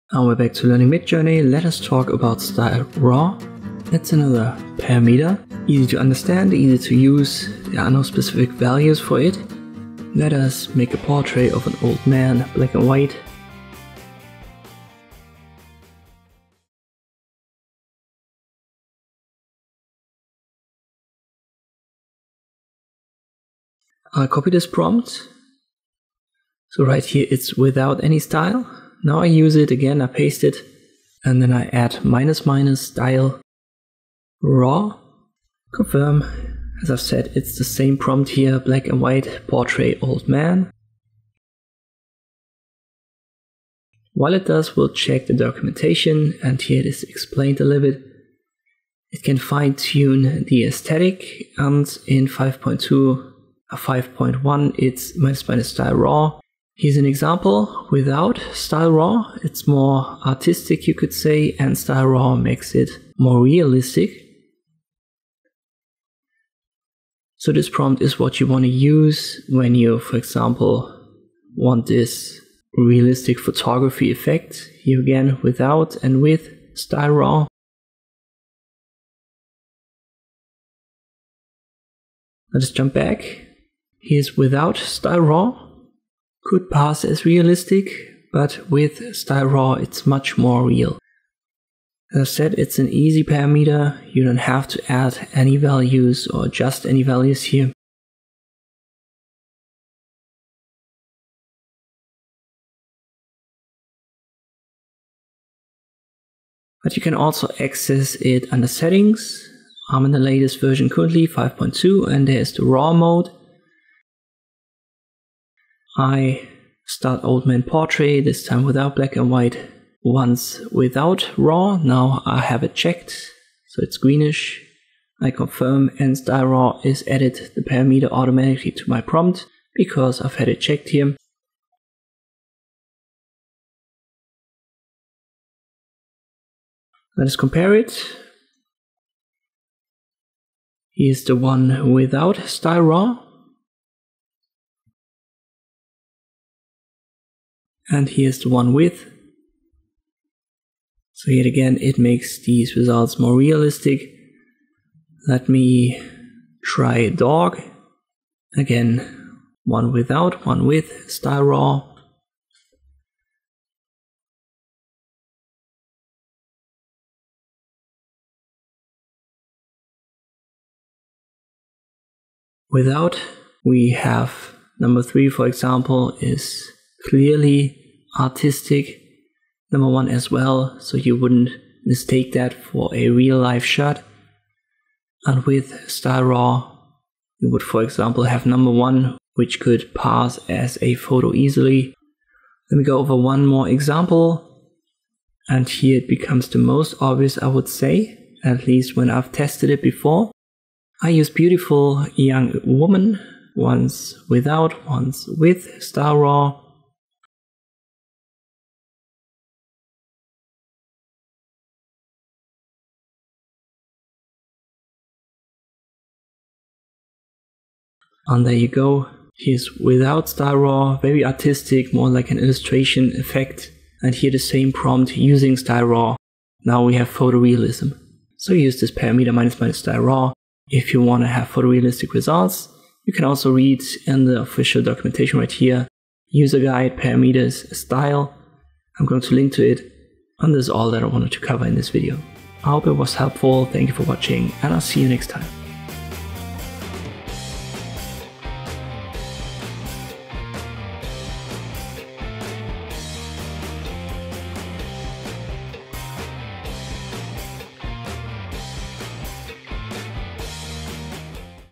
How to Use the Style Raw Parameter in Midjourney
In this tutorial I'll show you how to use the style raw parameter in Midjourney.